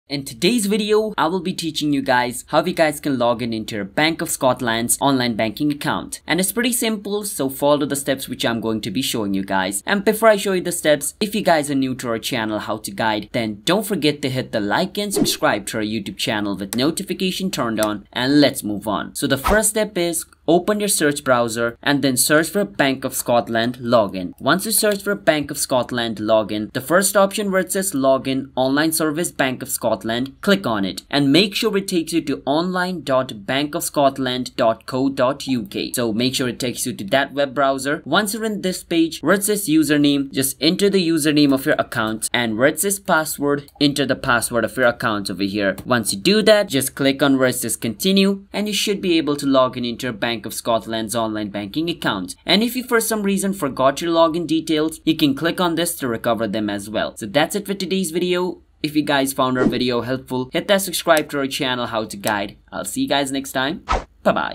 Bank of Scotland Login - How to Sign in to Bank of Scotland Online Banking Account (2023)
Confused about bank of scotland login? This video explains the exact steps on how to login bank of scotland online banking. In this easy-to-follow video guide, we'll walk you through the step-by-step process of accessing your Bank of Scotland online banking account. Whether you're a seasoned user or new to online banking, our comprehensive tutorial will ensure a smooth login experience.
Bank of Scotland's online banking platform offers a range of convenient features, and we'll show you how to securely sign in from any device, keeping your financial information safe and protected.